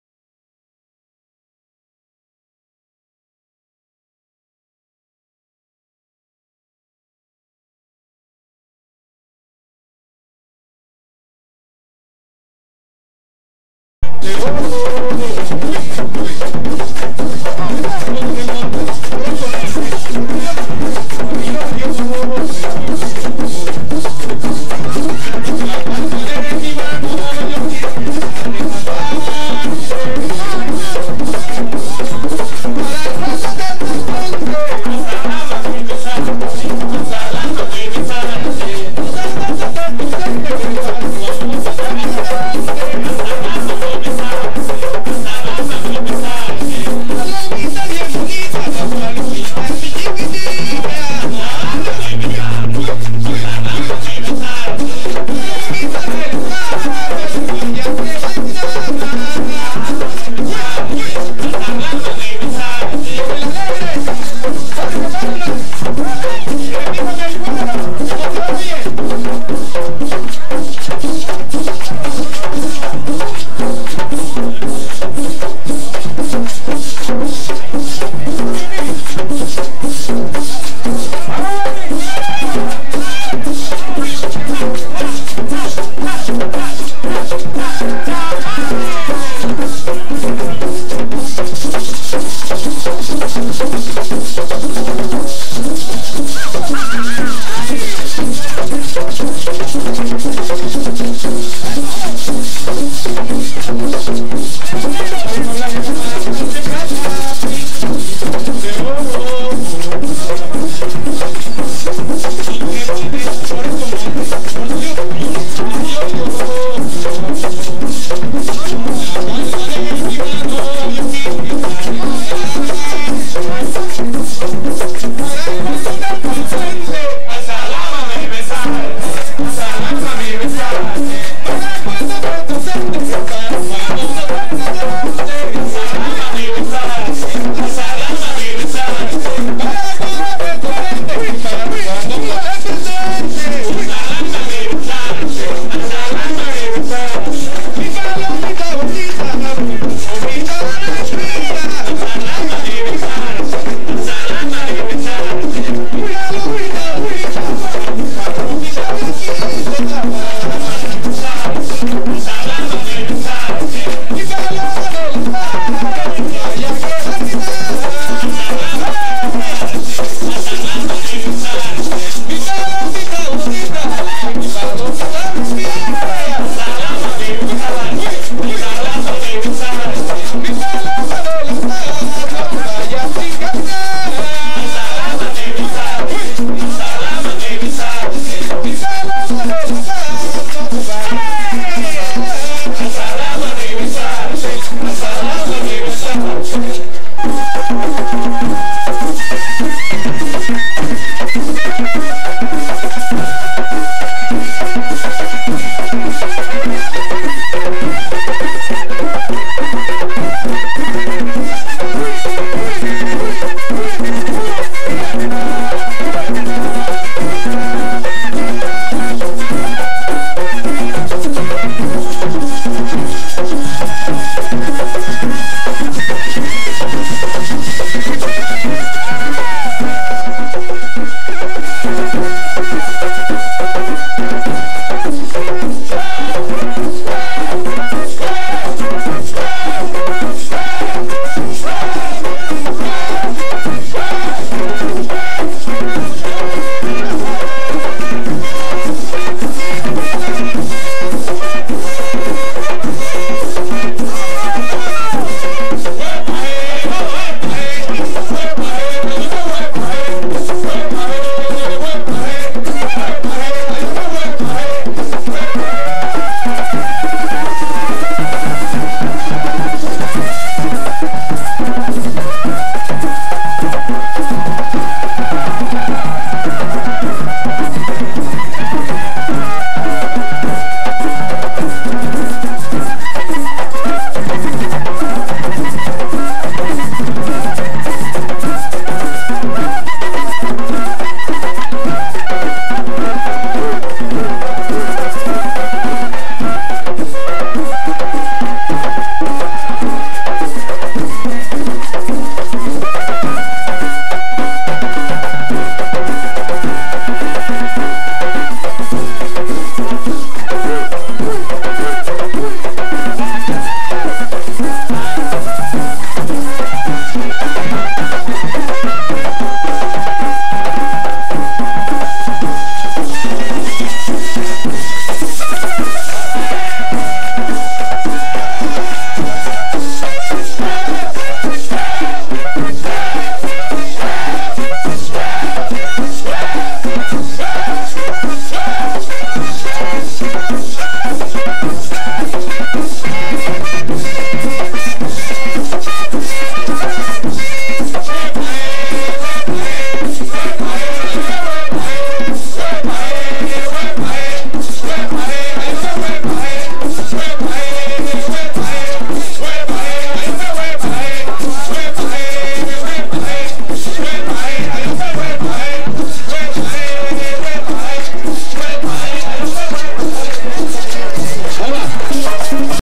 Cumbia tradicional en la Noche de Tambó, VIII parte, carnaval de Barranquilla, 2004
Imágenes de la participación de destacadas agrupaciones de cumbia tradicional en la ‘Noche de Tambó’,  el evento es considerado la rueda de cumbia más grande del Carnaval de Barranquilla, en la Plaza de la Paz, donde todos los asistentes bailan al ritmo de  la cumbia. La primera versión de la Noche de Tambó se realizó en 1995 y estuvo denominada como ‘Rueda de Cumbia al pie de una vara santa’. Con este evento, los hermanos Lisandro y Camilo Polo buscaban resaltar la esencia misma de las ruedas de cumbia y todo lo que en ella conlleva, por lo que se ha llevado a cabo desde ese entonces ininterrumpidamente. En sus versiones han contado con la participación especial de grandes artistas como Antonio Peñaloza, Efraín Medina, Mane Arrieta, Pedro Ramayá, Gabriel Segura, Aurelio Fernández, Esthercita Forero, Petrona Martínez, Adolfo Pacheco, Checo Acosta, Los Gaiteros de San Jacinto, Carlos Piña, entre muchos más. Participaron entre otros, Tomás Teherán Salgado, Amet Torres, Jorge Palma, Moris Cañate